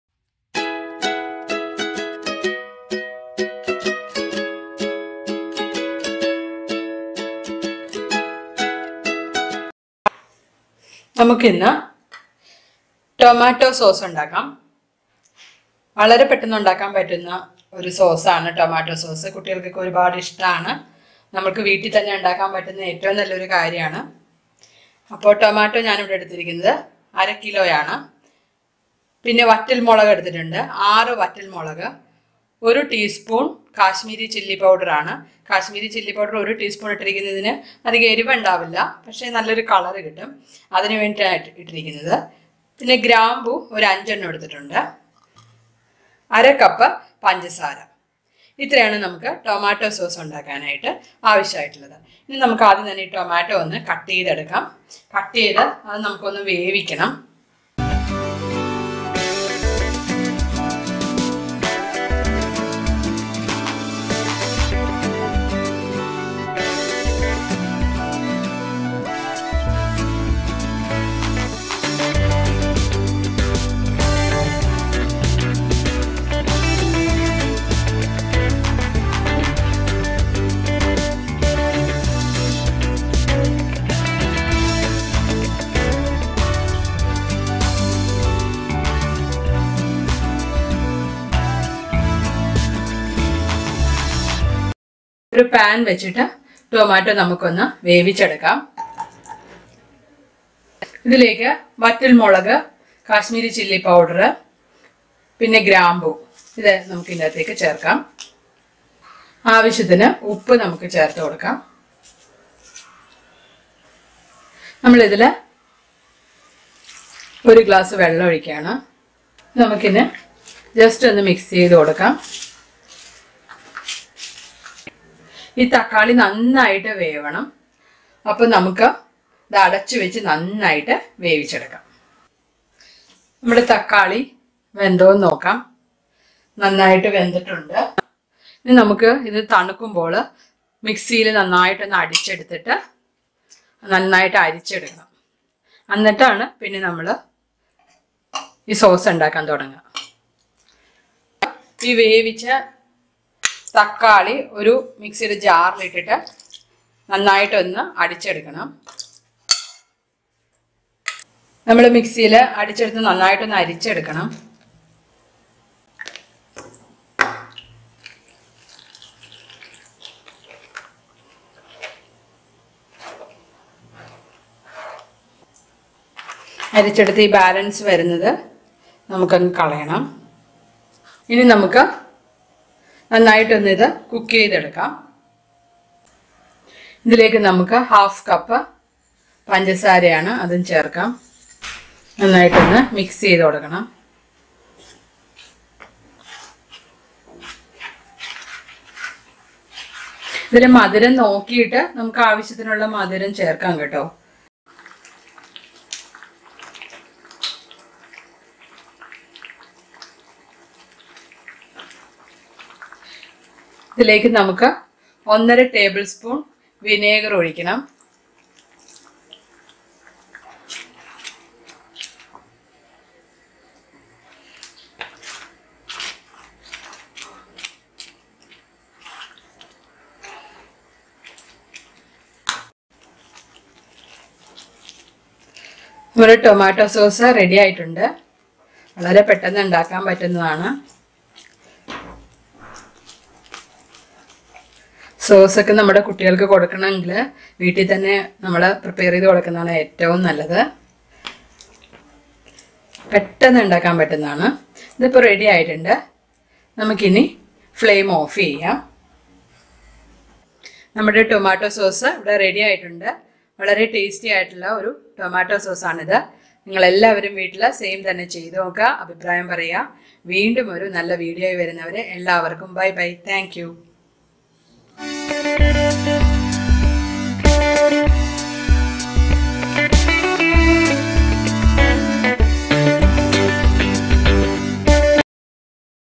TOMATO SAUCE/ TASTY AND HEALTHY TOMATO SAUCE AT HOME.
INGREDIENTS
TOMATO - 1/2 KG
SUGAR - 1/2 - 3/4 CUP
VINEGAR - 1 1/2 TBSP

PREPARATION
* firstly cook tomato pieces with red chilly, salt, cloves, chilly powder.
* switch off the flame when it cooked well. Keep aside allow them to cool.
*Make fine paste of cooked tomato .
* take a pan cook in medium flame add sugar vinegar and allow them more thick .
* Check the taste . If needed more sugar you can add.
* switch off the flame allow them to cool and transfer it to airtight container.